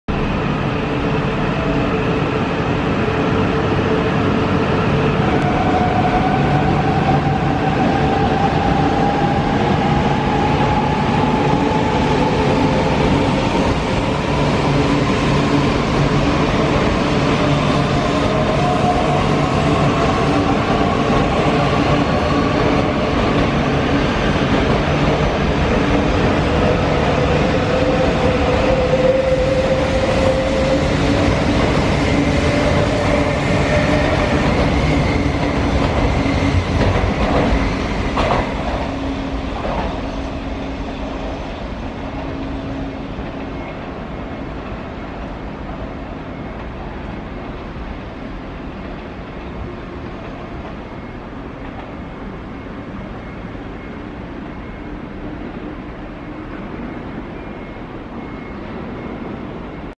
JR東日本【上越新幹線】E4系新幹線Max, Shinkansen E4 Series
2017年、JR東日本 - Max上越新幹線 - E4シリーズ新幹線高崎駅へ。
On 2017,  JR East Japan - Jōetsu Shinkansen E4 series "Max", depart from Takasaki Station.
2017年, JR東日本 - 上越新幹線 - E4系新幹線 Max, 高崎站開出.